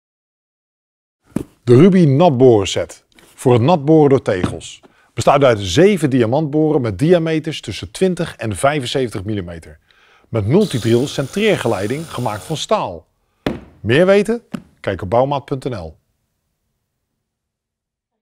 Rubi Natborenset easy gres plus Speedreview
De video toont een snelle review van de Rubi natborenset en hieronder is nog extra informatie te vinden.

De water druktank zorgt voor de koeling tijdens het boren. De kwaliteit van het diamant maakt boren in aardewerk, porselein, graniet, marmer en glas mogelijk. Het gebruik van de Multidrill centreer geleiding zorgt ervoor dat de boor tijdens het boren niet beweegt. De boren hebben een zijdelingse opening voor een betere koeling en het gemakkelijk verwijderen van het boorgruis.

De set bestaat uit een universele boorgeleiding, boren ø 20, 28, 35, 43, 50, 65 en 75 mm, watertank en slang. Wordt geleverd in een koffer.